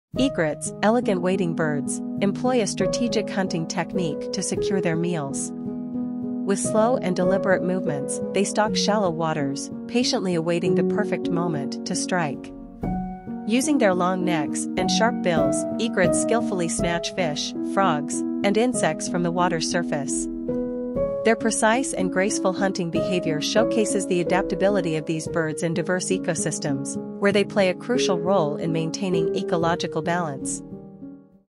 Egret Birds Hunting
Egrets, elegant wading birds, employ a strategic hunting technique to secure their meals.           With slow and deliberate movements, they stalk shallow waters, patiently awaiting the perfect moment to strike. Using their long necks and sharp bills, egrets skillfully snatch fish, frogs, and insects from the water's surface. Their precise and graceful hunting behavior showcases the adaptability of these birds in diverse ecosystems, where they play a crucial role in maintaining ecological balance.